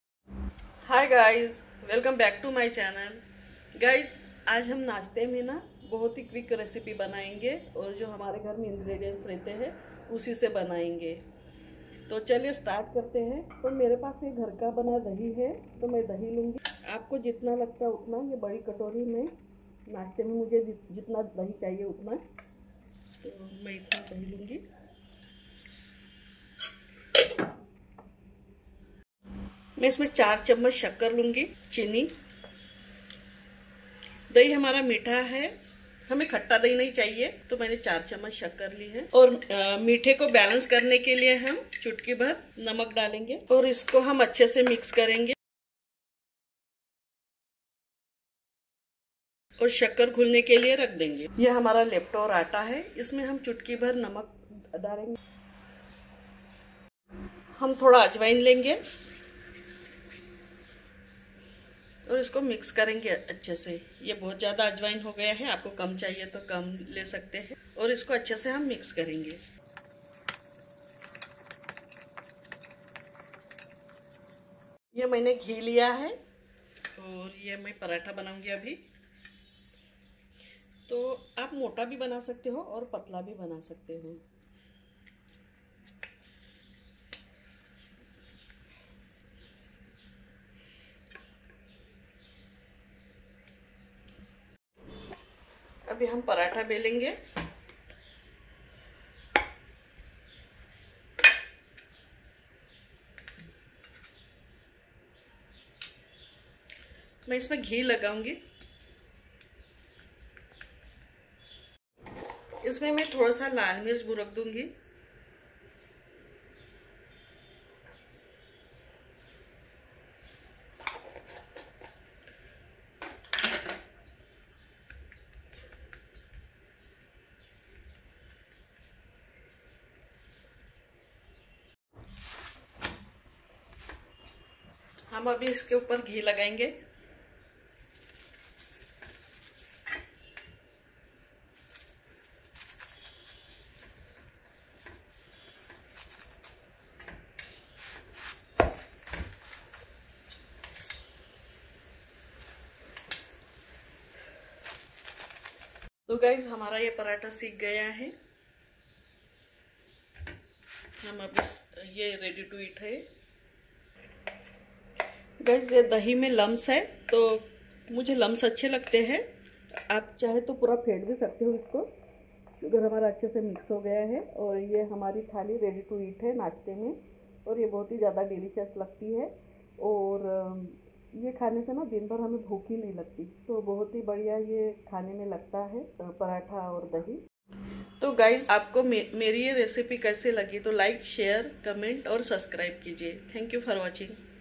How to Make Delicious Paratha with Dahi | दही के साथ एकदम टेस्टी पराठे की विधि
Looking for a new and exciting way to enjoy Paratha? Look no further than this recipe for Delicious Paratha with Dahi! In this video, I'll show you how to create a perfect balance of flavors and textures by combining soft, buttery Paratha with creamy and tangy Dahi. The result is a mouth-watering dish that's sure to impress. Whether you're a beginner or an experienced cook, you'll love how easy and fun it is to make this recipe. So gather your ingredients and let's get started!